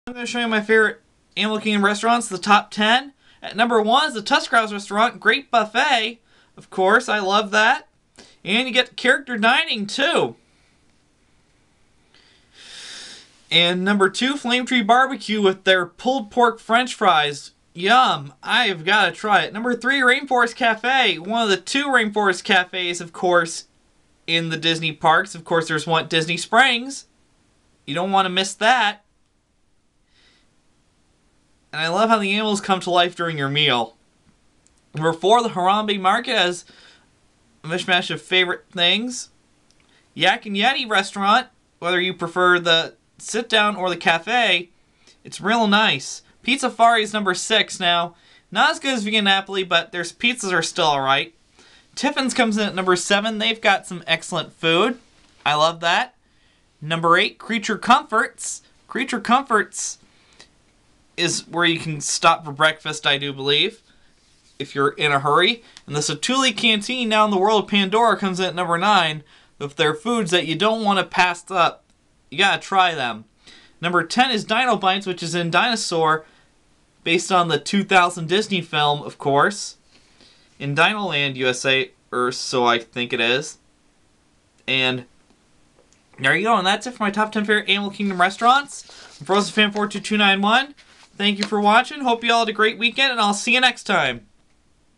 My Top 10 Favorite Animal Kingdom Restaurants
Here's A list of my top 10 Favorite Animal Kingdom Restaurants at Walt Disney World! and you will notice here that i do have some quick-service restaurants here too. i hope you like it! see you for more videos!